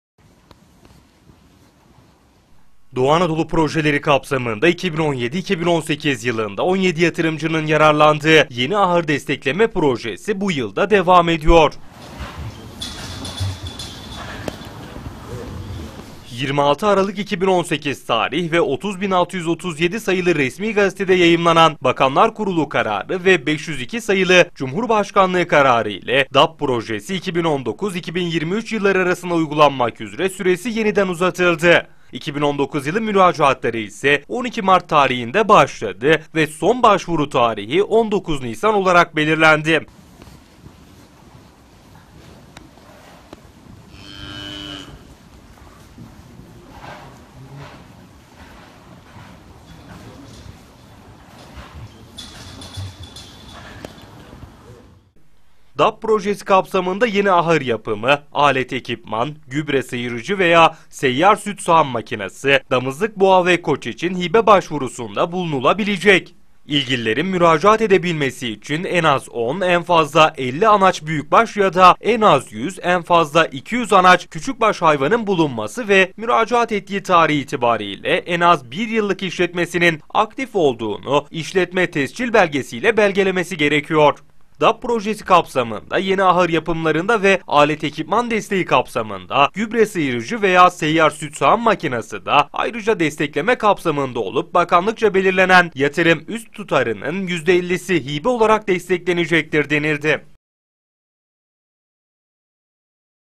YENİ AHIR PROJELERİ İÇİN SON BAŞVURU TARİHİ 19 NİSAN
DAP projeleri kapsamında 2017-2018 yılında 17 yatırımcının yararlandığı yeni ahır destekleme projesi bu yıl da devam ediyor.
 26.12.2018 tarih ve 30637 Sayılı Resmi Gazetede yayımlanan Bakanlar Kurulu Kararı  ve 502 Sayılı Cumhurbaşkanlığı  Kararı ile DAP Projesi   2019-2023 Yılları arasında uygulanmak üzere süresi yeniden uzatıldı. 2019 Yılı müracaatları ise  12 Mart tarihinde başladı son başvuru ise 19 Nisan.